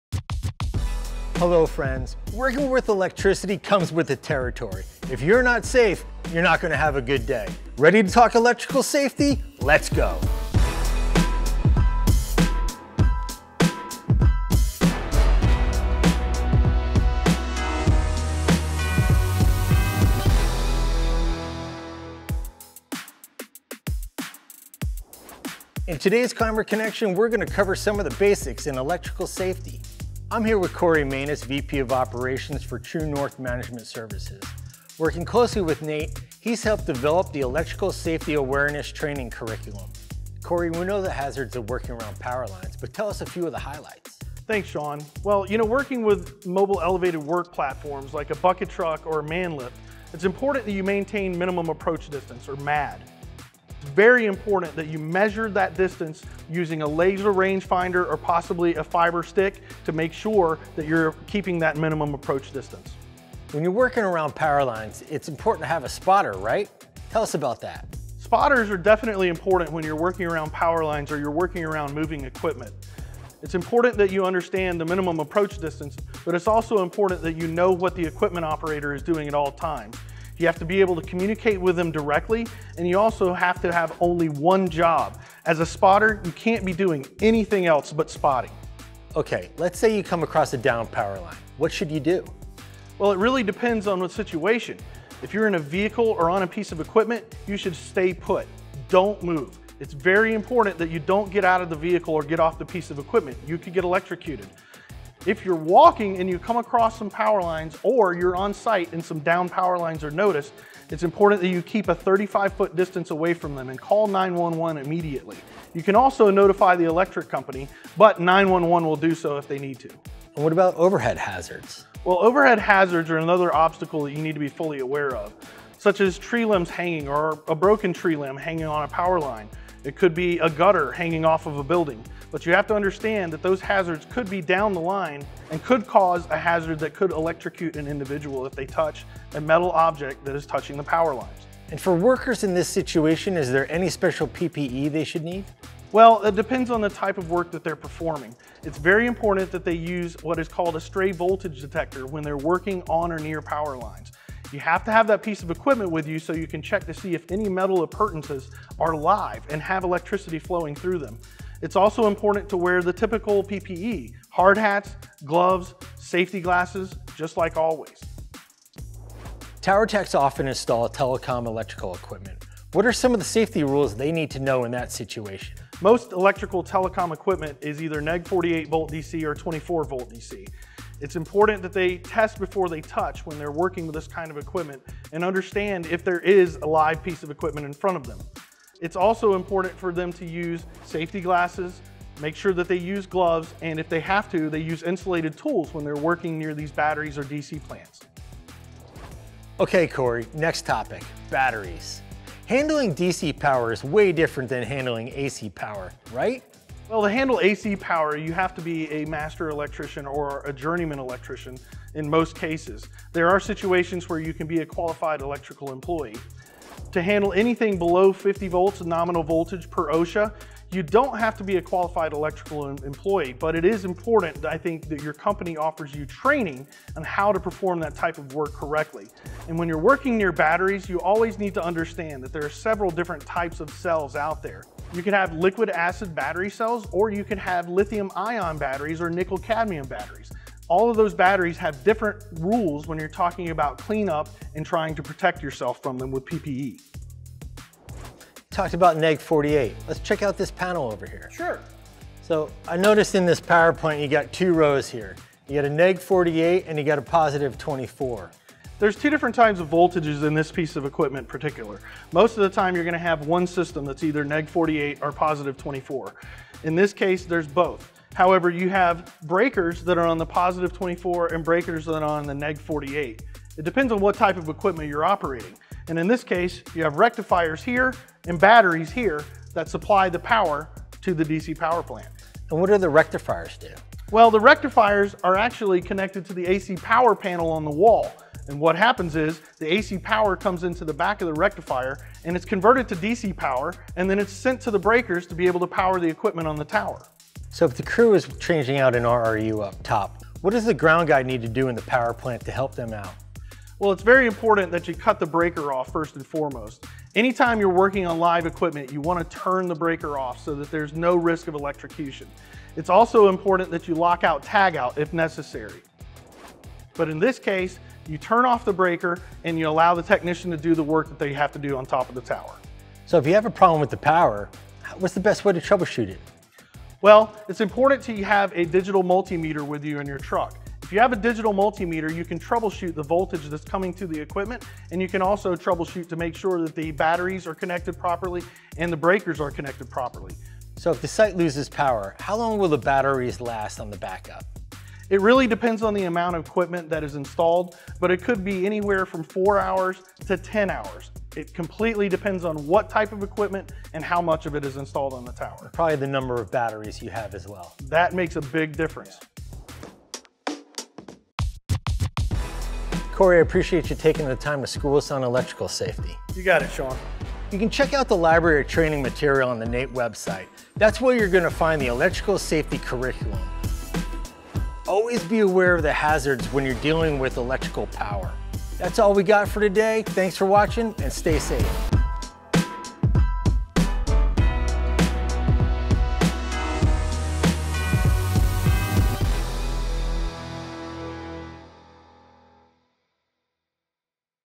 Electrical Safety NATE ClimberConnection Video
NATE: The Communications Infrastructure Contractors Association today unveiled a video shining a spotlight on the topic of Electrical Safety Awareness. The video is the second to Debut in Volume 7 of the Association’s popular ClimberConnection Series.

The NATE Electrical Safety Awareness video is intended to provide guidance to the industry’s technician workforce regarding the hazards associated with working near power lines as well as the equipment and training protocols necessary to keep workers safe in these environments. The video also highlights the NATE Electrical Safety Awareness training resources that are available for free to the Association’s members and stakeholders in the industry.

“As convergence happens and the industry continues to evolve, we are seeing more scenarios where tower technicians are getting exposed to overhead and downed electrical hazards while deploying wireless and broadband services,” said Corey Manus, Vice President Business Development and Special Projects at TES, a Congruex Company. “The video touches on many crucial elements the industry’s workforce needs to be educated and trained to ensure their safety when working in these environments. I would strongly encourage companies in the industry to incorporate this video into their tailgate safety meetings and training sessions,” added Manus.

The ClimberConnection Volume 7 campaign is designed to supply education on timely topics directly to the industry’s workforce.